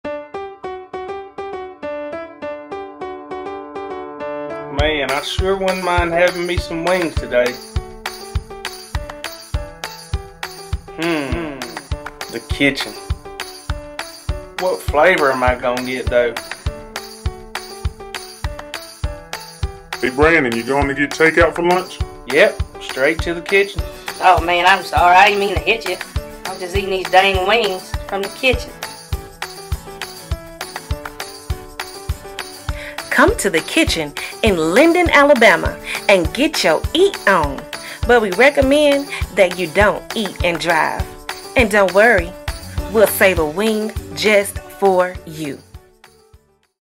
The Kitchen Commercial
Come get you some wings!!!
Sundays 12pm-6pm, Mondays-Wednesdays 11am-11pm, Thursdays 11am-2pm, Fridays 11am-1am and Saturdays 1pm-1am